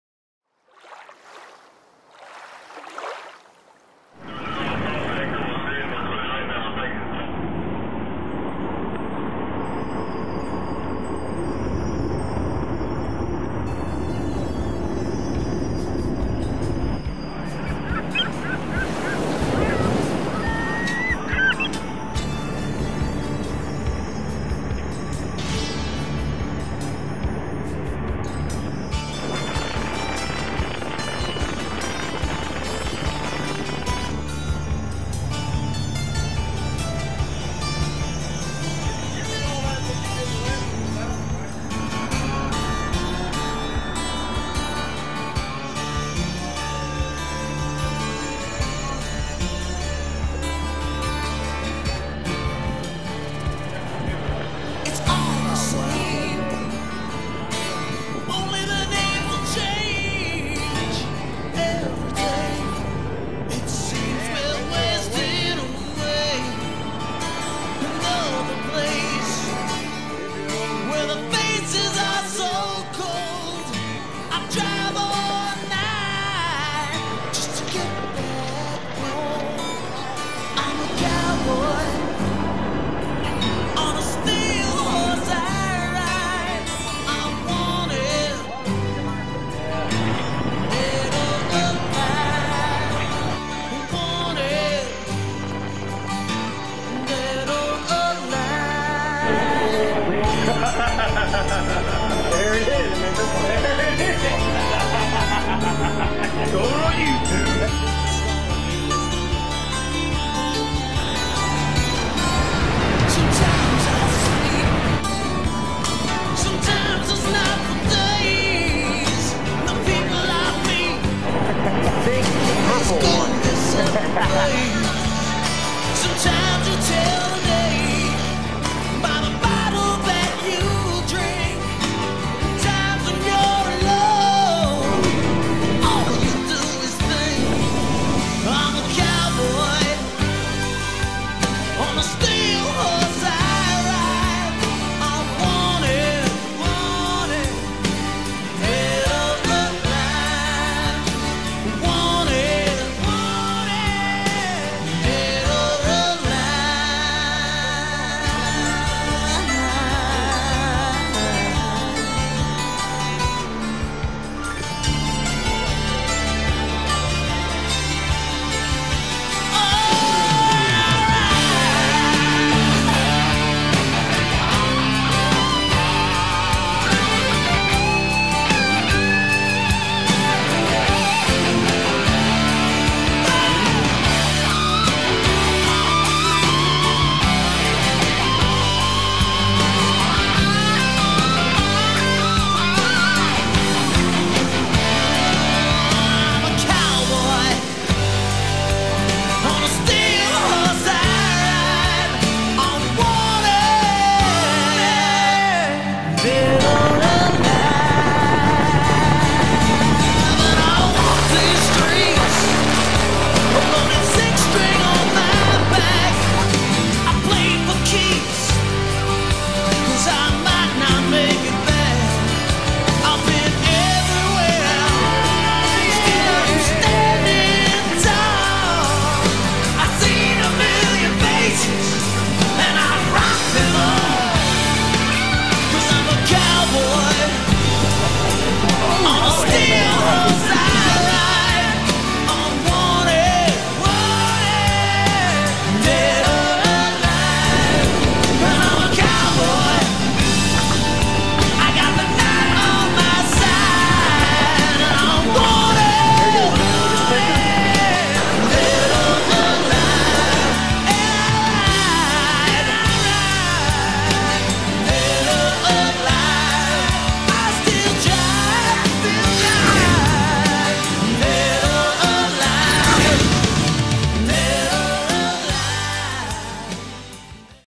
Drillin & Billin
Aboard Ed Amos' Drillin & Billin.  2010 White Marlin Open and Mid Atlantic 500, Ocean City, MD.  with Captain Jack and the crew...

2011 Season begins May 11th with the Hatteras Village Open